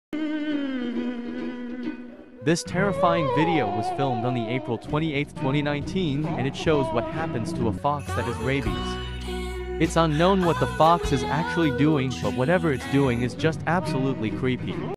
What The Effects Of Rabies Does To An Animal
In this video I'll show you guys what the effects of rabies does to an animal.